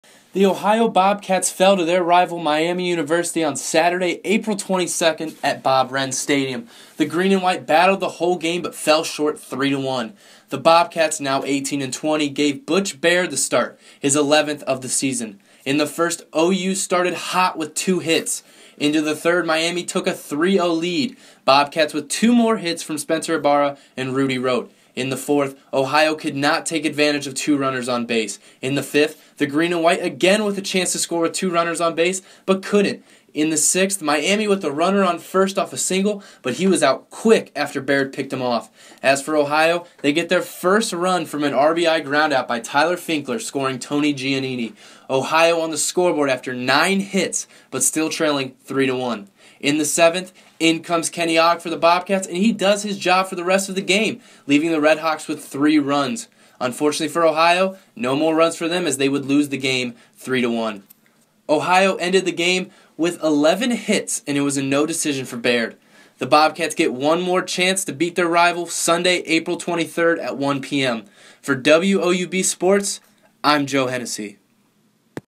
Ohio Baseball: Ohio loses to Miami University in Close Battle
WOUB Reporter Joe Hennessy recaps the Bobcats lose against their rival, Miami University, 3-1.